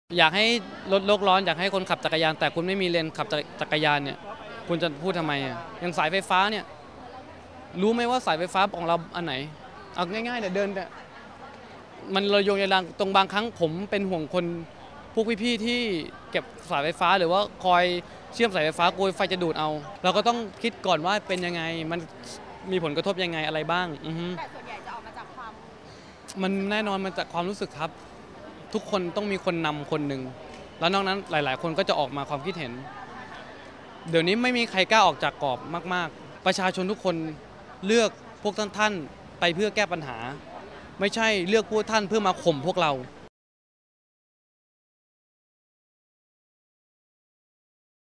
แท๊คกับมุมมองทางการเมือง
มุมมองของคนบันเทิงอีกหนึ่งคนที่กล้าจะพูดความในใจแบบตรงไปตรงมา

คลิปข่าวจากช่อง Super บันเทิง True 70 และ 38 // Psi 38 // ipm 45 // GmmZ 83